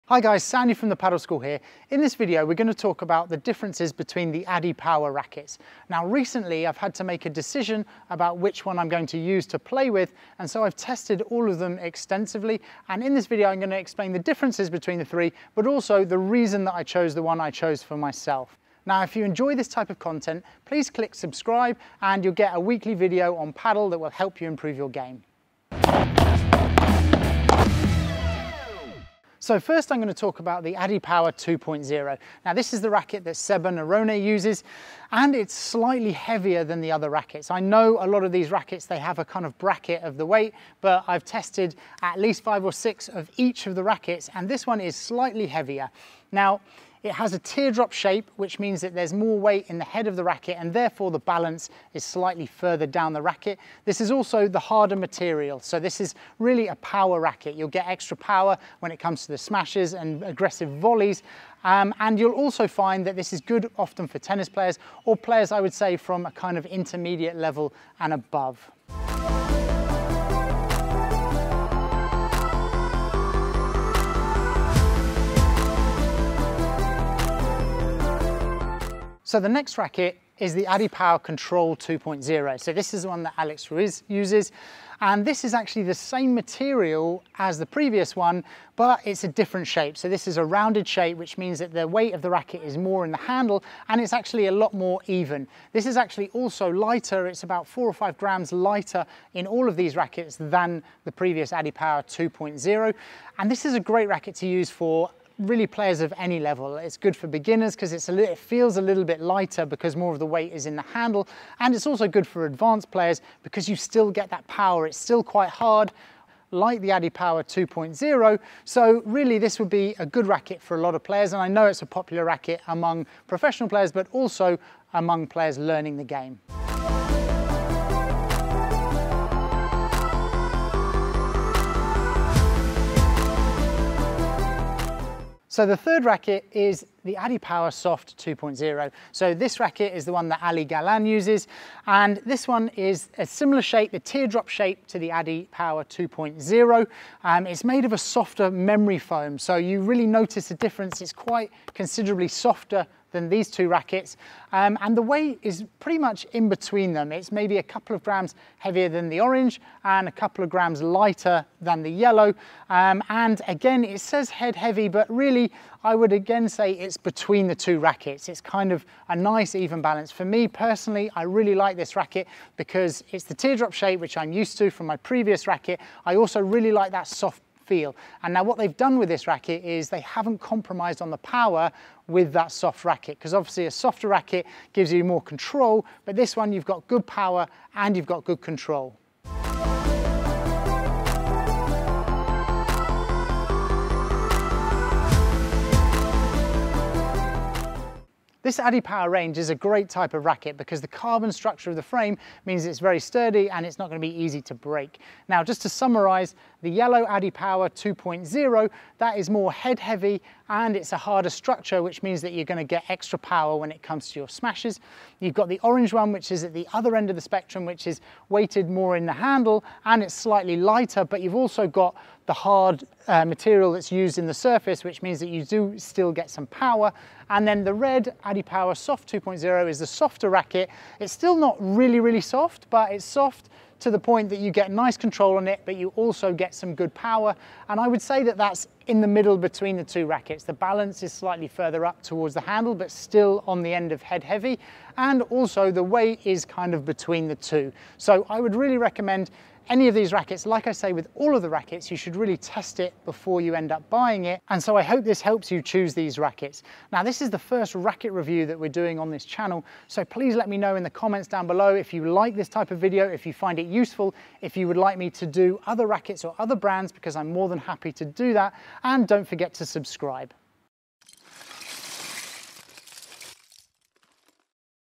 ADIPOWER Padel Rackets COMPARISON - Adidas Padel
In this video I compare the difference between the Adidas padel rackets! These 3 high quality models from adidas have different characteristics that will suit different players!

Here's an article on buying your first racket and how to save money

These are some excellent quality padel rackets and hope you choose the one that suits you best!